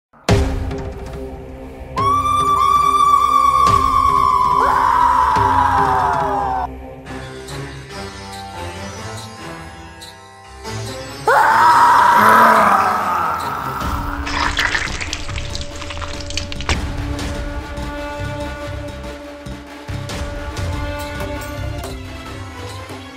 Mahakali trailer I Bengali Animated Video I Indian Animation I Creative Animation
We are Creative Animation, an animation team from Kolkata. As the Kali puja festivities starts, so this video has been made dedicated to the power of goddess Maa Kali. Please support us.